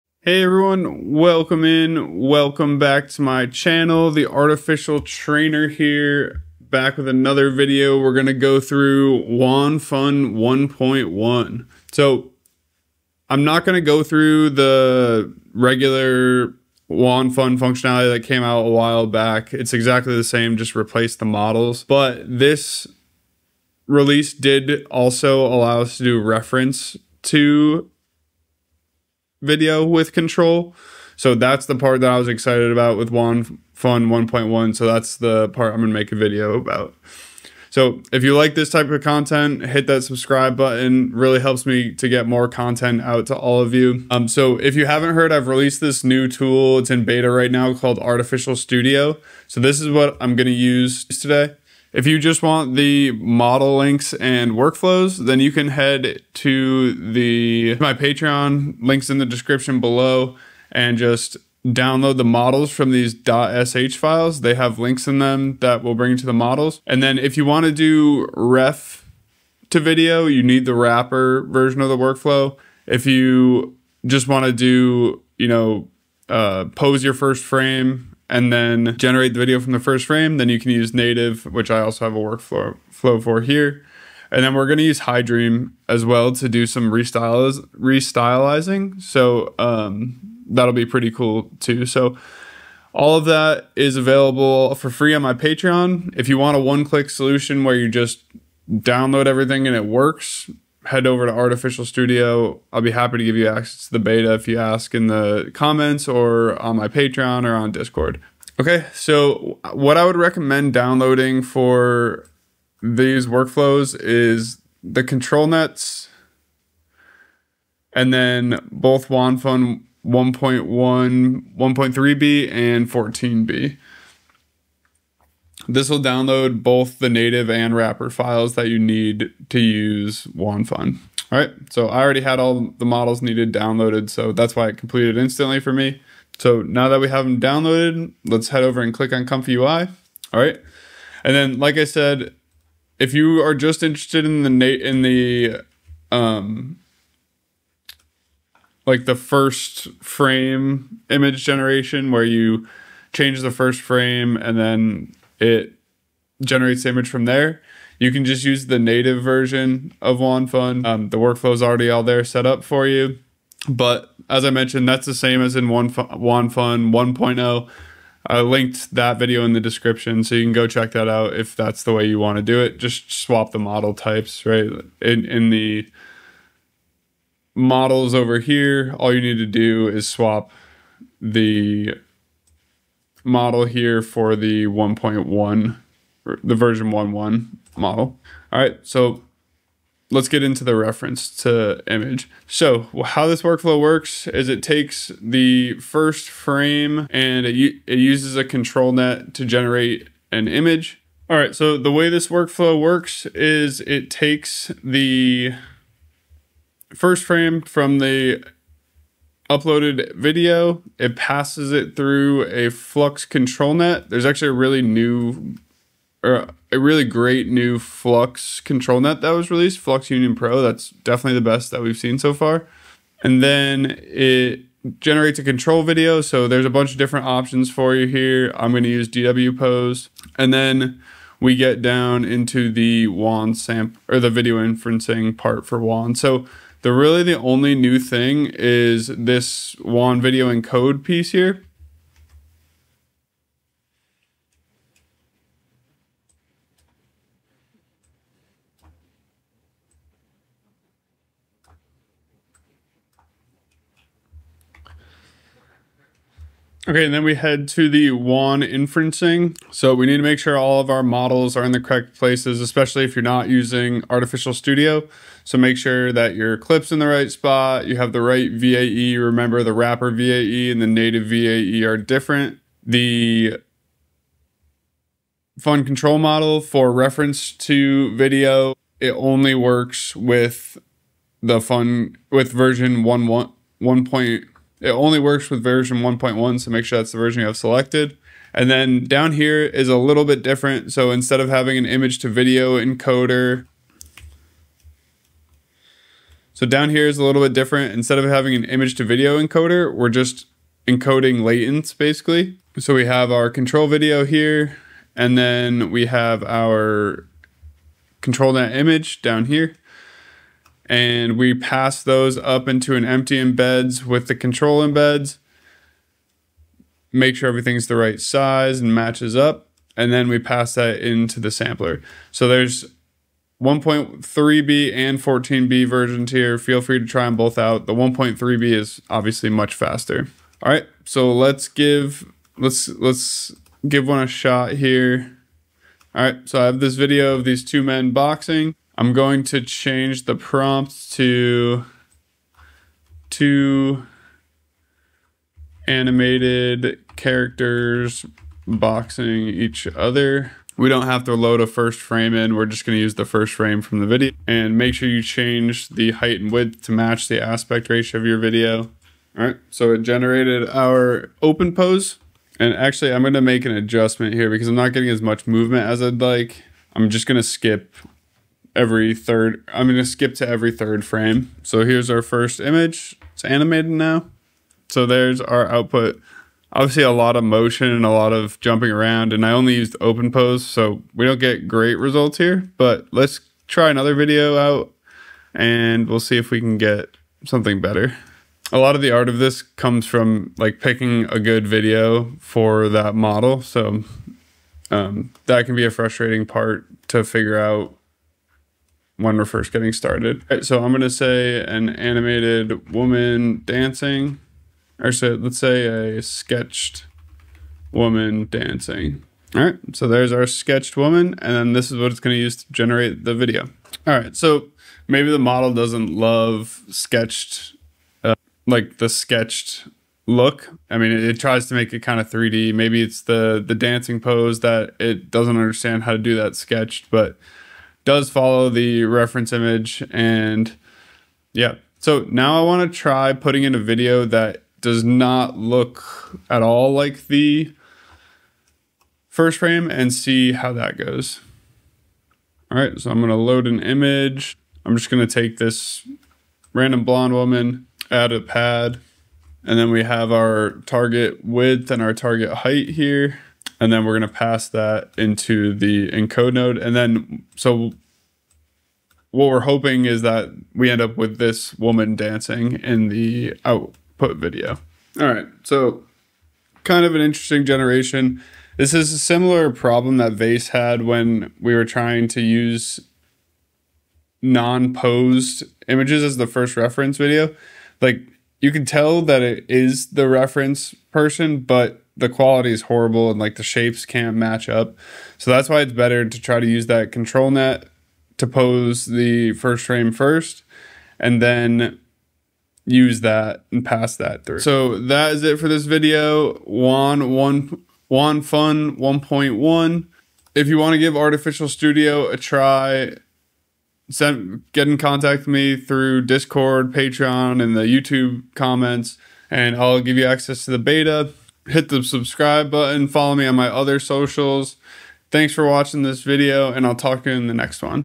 Wan Fun 1.1! Improved AI Video Quality + Reference 2 Video! Workflows + Tutorial
In this video, I showcase WanFun 1.1, the groundbreaking update that makes AI-powered animation easier than ever—even with unposed reference images. Whether you're working with a still photo or a casual reference, this tool lets you create dynamic, realistic motion with almost no effort. I'll walk you through the entire setup, from model downloads to finished video demos—including a boxing animation, a dancing woman, and an unposed-to-video workflow that’s perfect for beginners and pros alike. If you’ve been searching for the ultimate animation workflow for beginners, this is the one to watch.

⏱️ Timestamps:
0:00 Intro
0:50 Download Models + Workflows
3:08 Workflow Explained
5:30 Boxing Demo
6:25 Woman Dancing Demo
7:15 Unposed Reference 2 Video
8:15 Outro